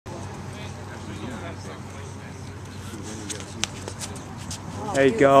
Alex - Discus at Holder Relays 03/30/2013
Track and Field practice and meet videos for Cedarcrest High School throwers.